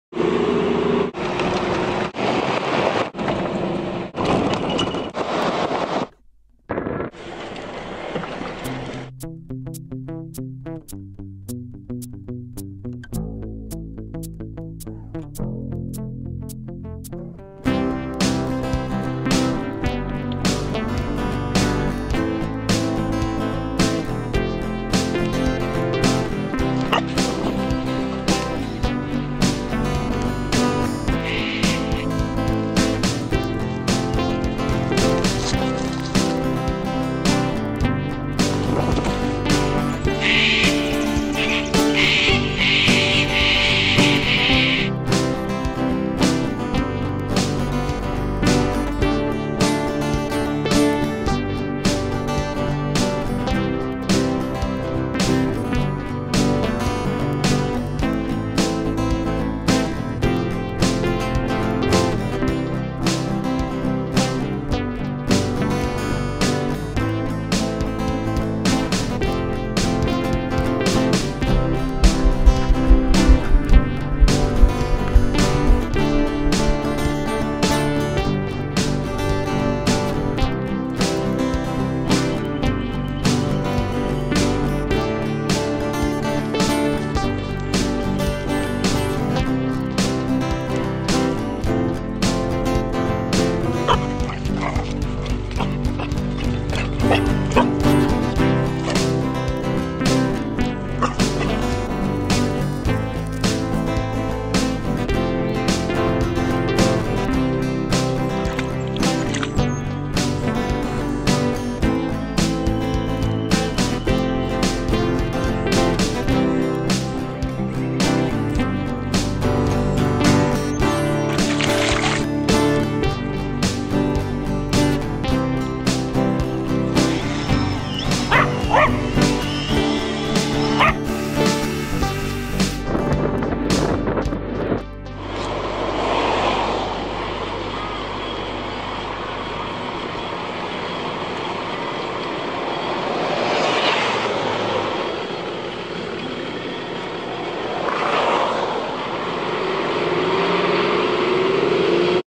Zuki at the Murray river
Last minute camping trip with Zuki our 7 month old Kelpie